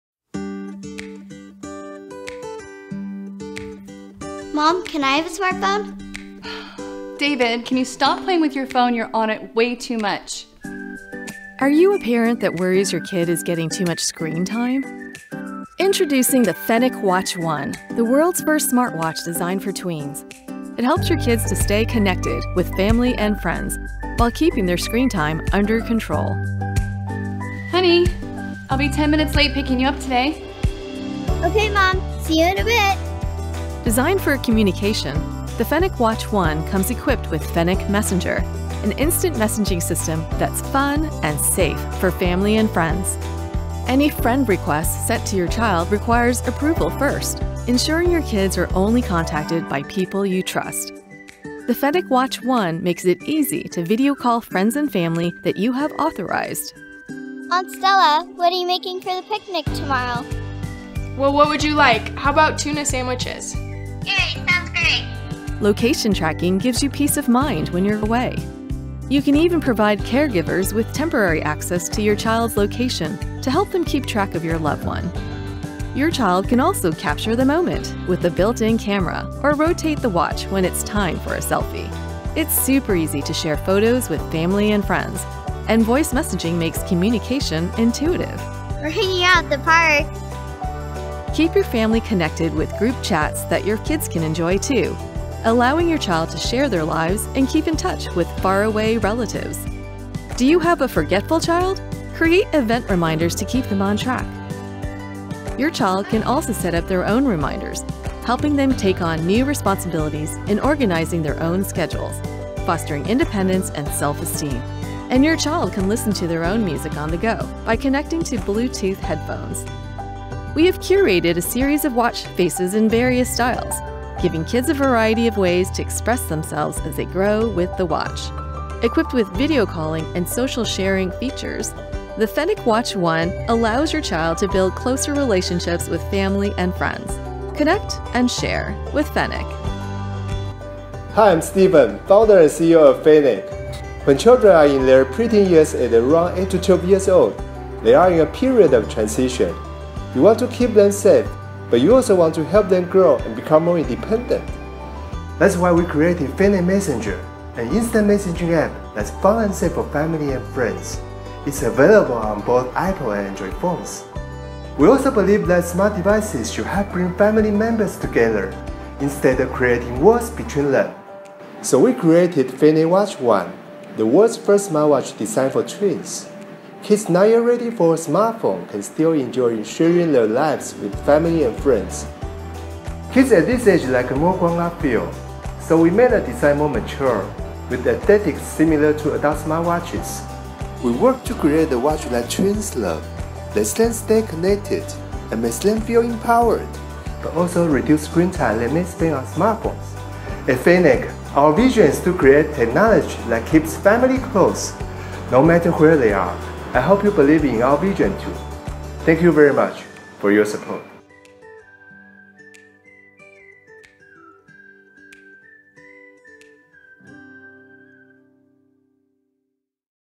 FENNEC WATCH: THE FIRST SMARTWATCH DESIGNED FOR TWEENS | IndieKickZone
A cool watch that connects kids, families, and friends.